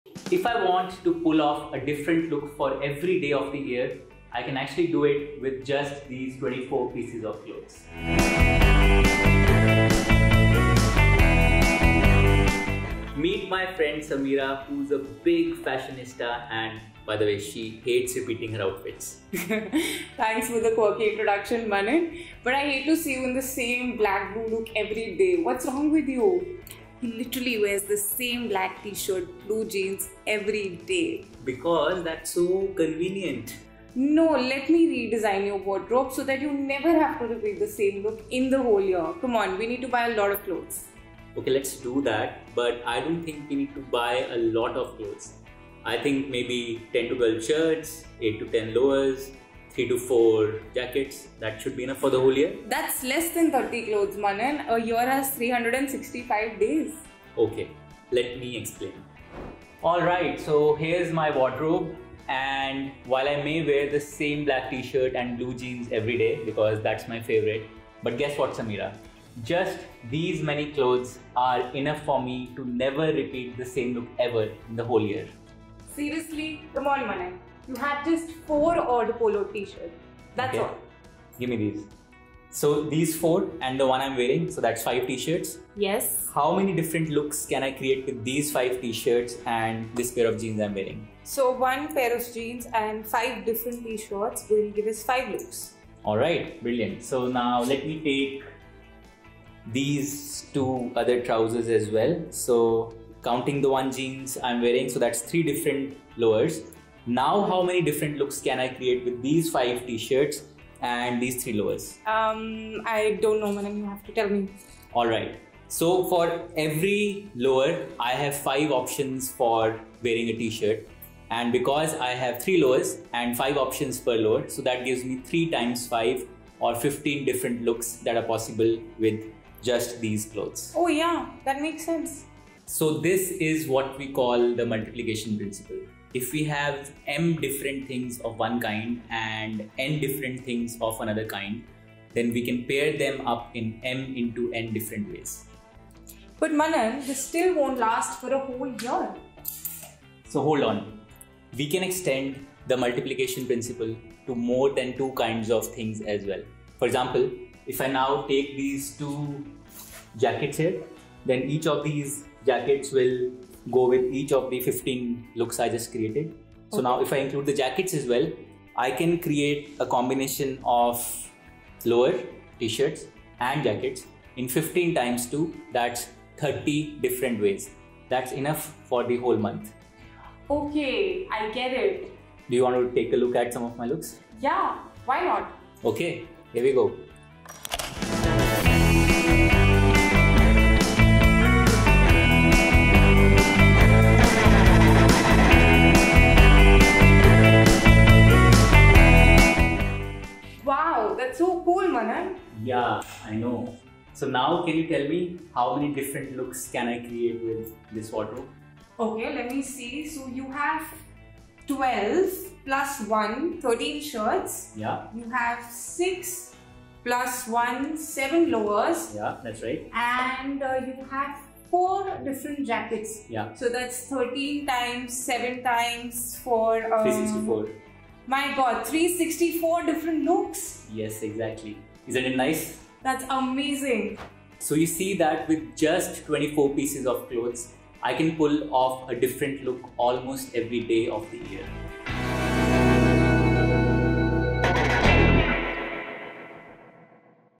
Never Repeat the Same Look 👕
How many pieces of clothes would you need to pull off a different look everyday for the entire year?

Find out as we explore one of the fundamental principles of counting!